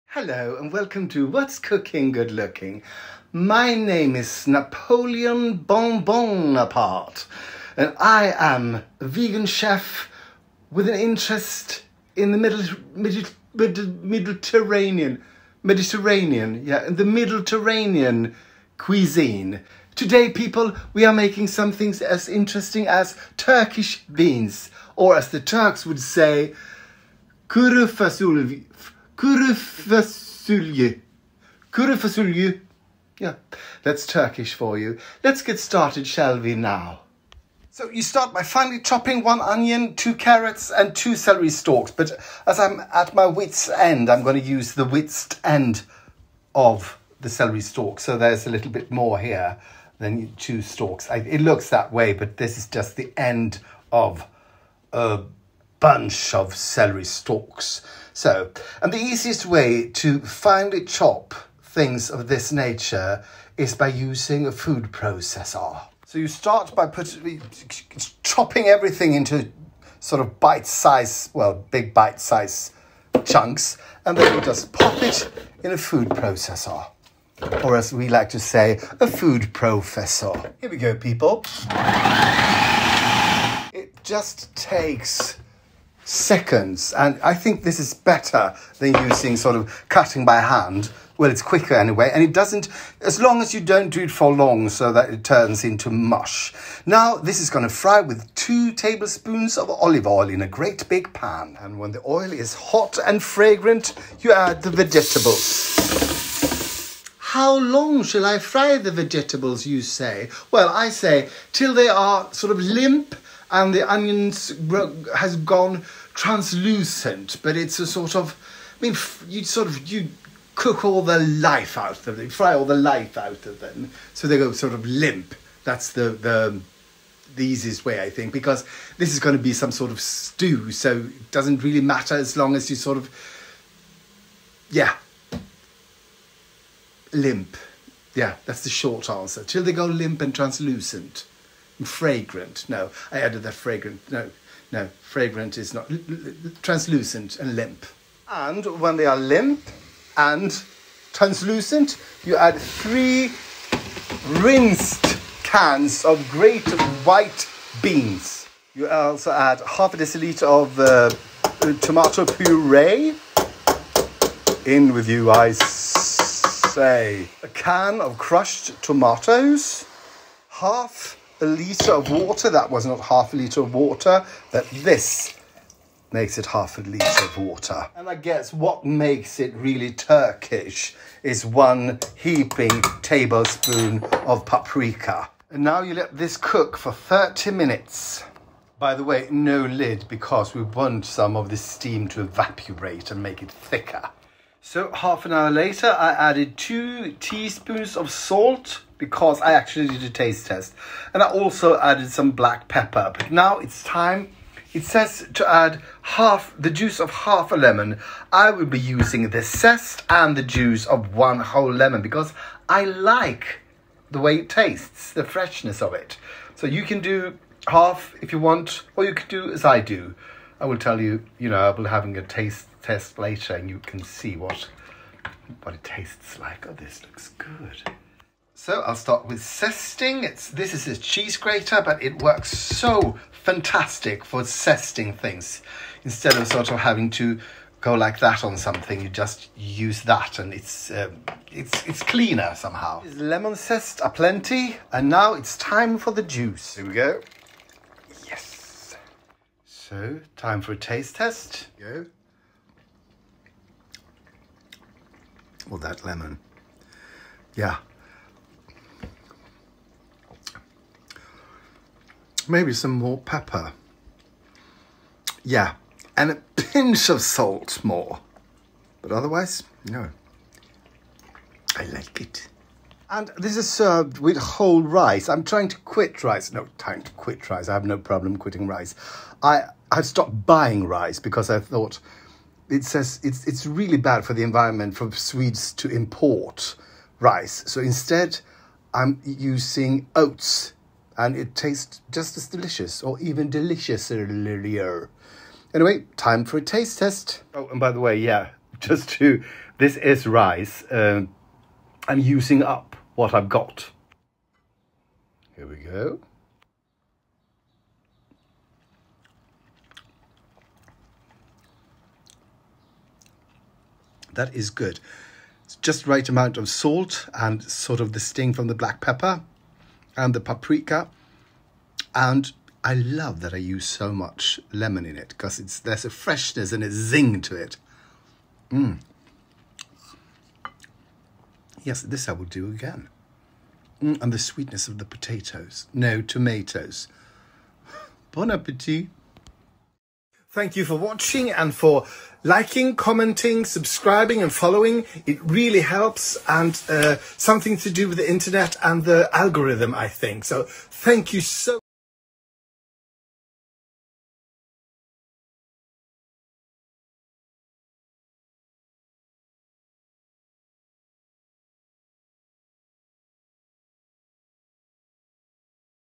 HOW I MAKE SEXY KURU FASULYE SEXY TURKISH BEANS
So, I'm not en expert in turkish cuisine...